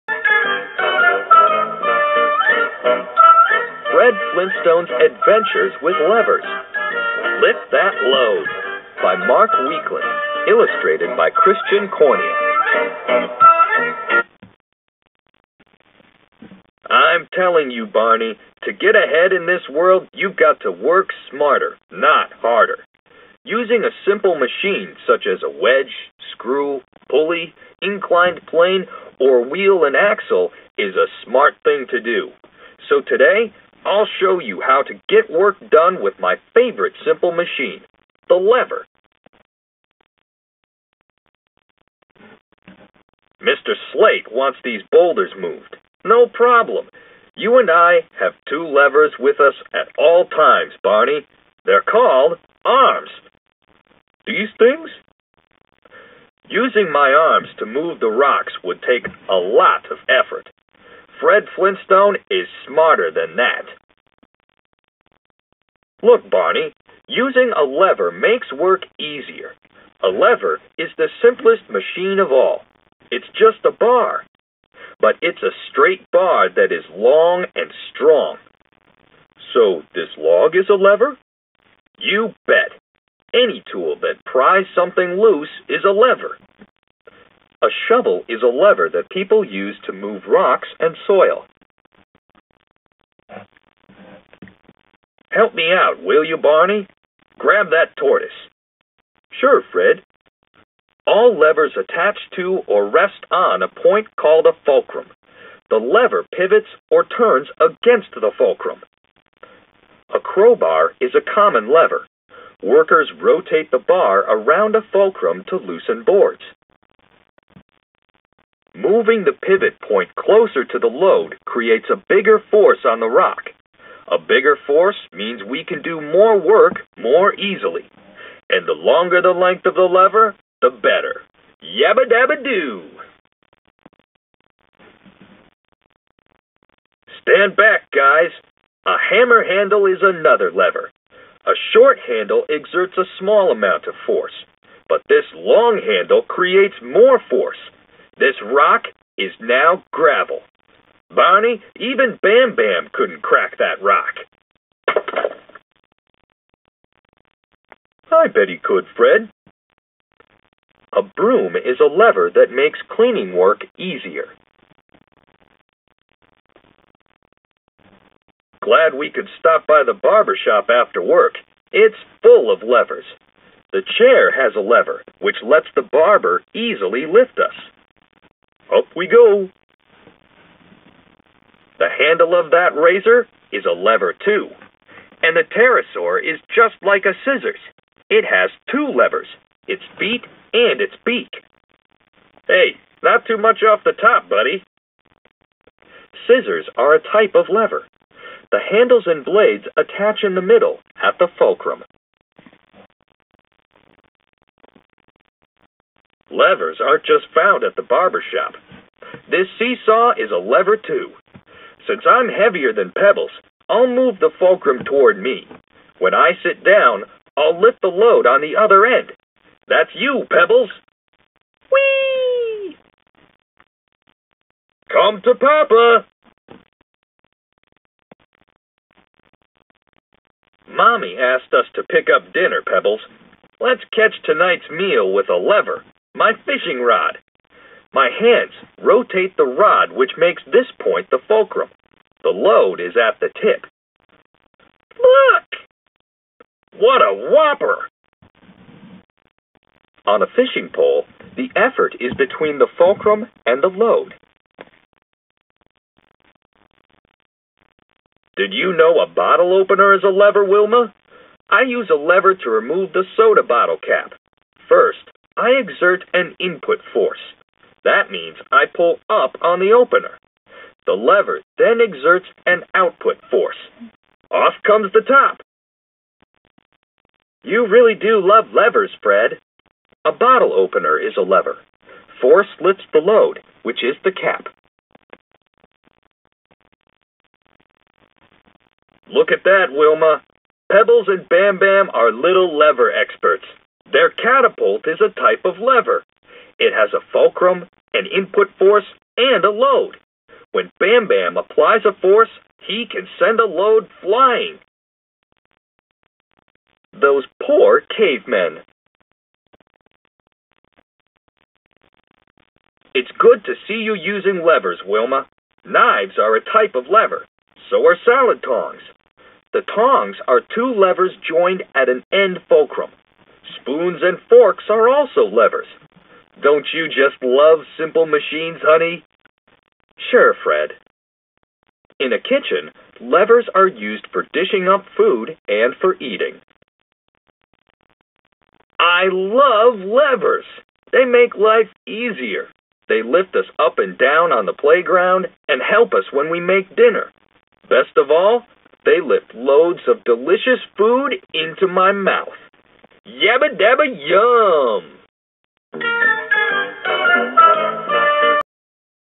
Fred Flintstone's Adventures with Levers
Have a Yabba-Dabba-Doo day with Fred Flintstone as he explains how a lever works! Fred Shows readers the Flintstone way of using simple machines in everyday situations. With simple,informative  text narrated by Fred and a few other Flintstone characters paired with colorful illustrations, students will learn all about simple machines and basic physics concepts!

Recommended Grade Levels: KG-2
                                 Language : English
                                       Author : Weakland, Mark
                                  Illustrator : Cornia,Christion
                                   Publisher: Capstone Press
                               Book series: Flintstone explain simple machines
                                          Lexile: 500L